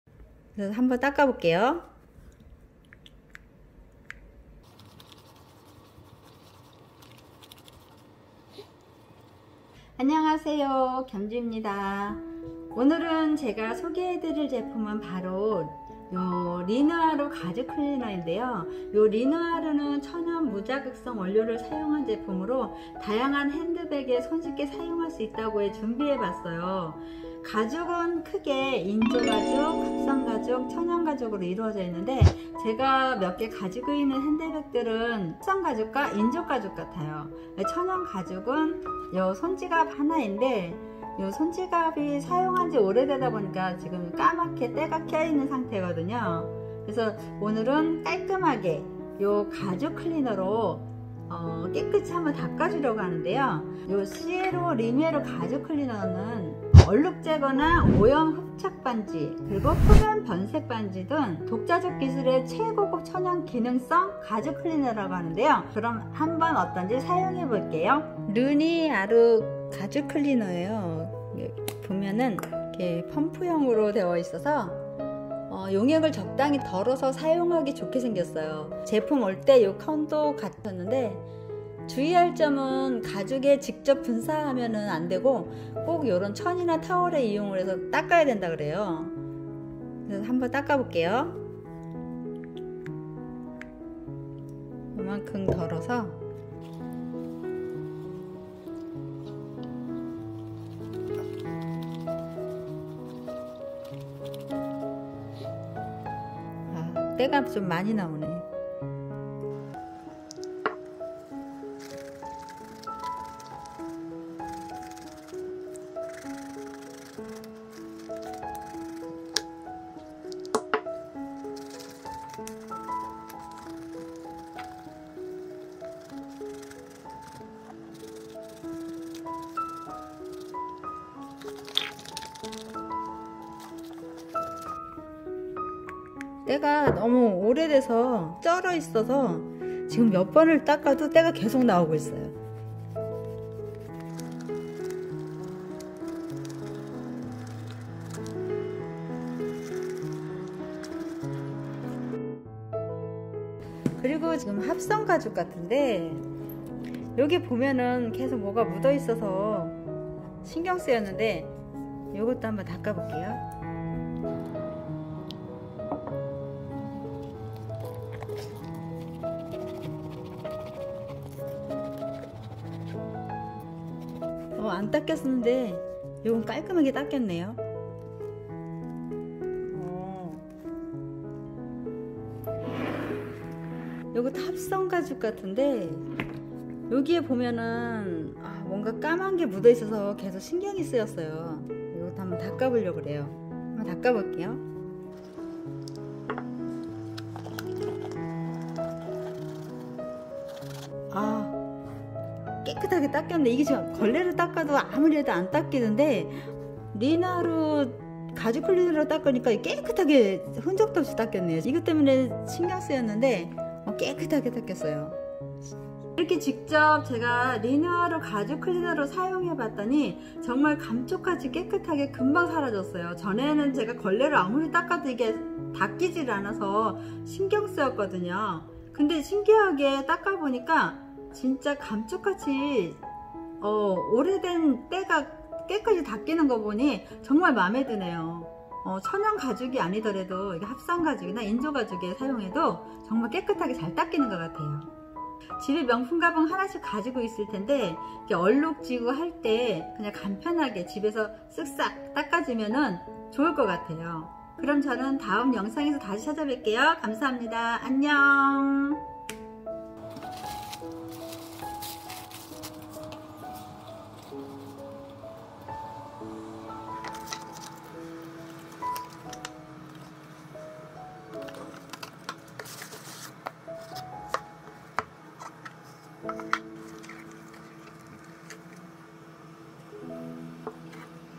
가죽클리너 뤼느아르 가방 얼룩 잘지워지네요!
가죽가방 하나씩은 다 가지고 있지요~
얼룩 묻었을때 참 속상할때 많은데
이제 그럴땐 가죽클리너 뤼느아르르로 싹싹 닦아주세요~!

가죽크리너 뤼느아르 자세한정보는 아래에서 확인바래요

'이 영상은 업체로부터 이용권을 제공받아 솔직하게 제작되었습니다.'

감사합니다.